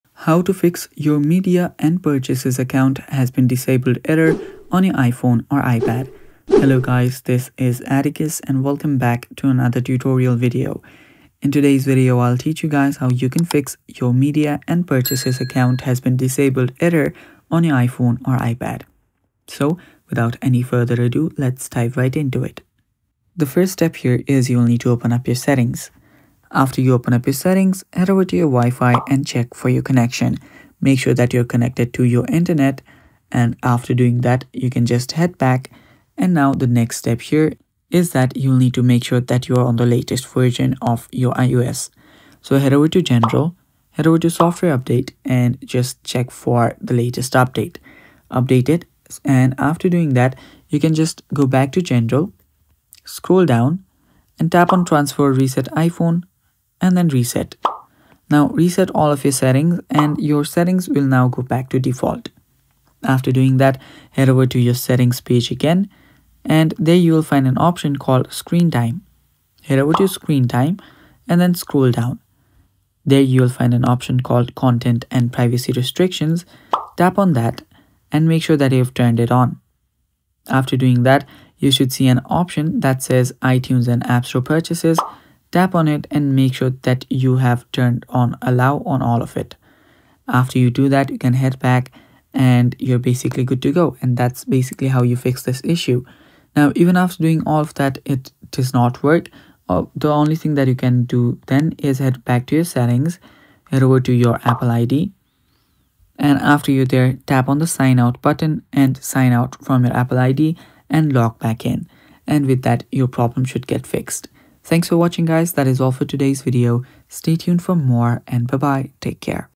How to Fix “ Your Media and Purchases Account Has Been Disabled” on iPhone
How to Fix "Your Media and Purchases Account Has Been Disabled" on iPhone (2025)!
Is your iPhone showing the message "Your Media and Purchases Account Has Been Disabled"? Don’t worry, this video will guide you through the steps to fix the issue and regain access to your account. Whether you're wondering, "Why has my media and purchases account been disabled?" or looking for a quick solution, this tutorial has all the answers. Learn how to resolve the issue and continue using your Apple services. Watch now to fix your disabled account on your iPhone today!

Timestamps:

00:00 How to Fix "Your Media and Purchases Account Has Been Disabled"
00:10 Why Is My Apple Account Disabled and How to Fix It?